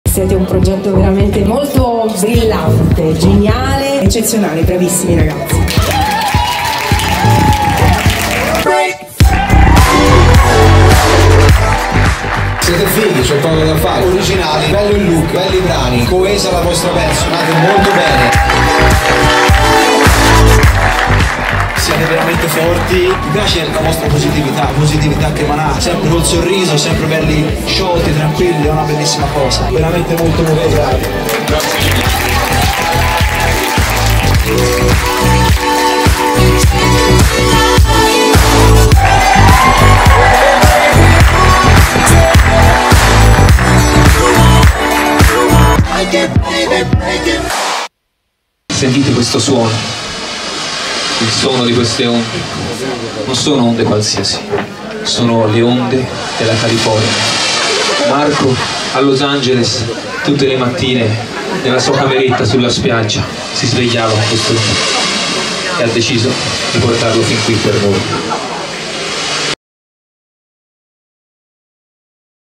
BAND LOCALE CONQUISTA IL LOCALE: LA FLEXATA
Video indiementicabile in cui delle voci ci fanno dei complimenti. Sembriamo seri e non è cosa da poco. D'ora in poi sarà il nostro curriculum, che useremo per conquistare le bettole di tutta la penisola.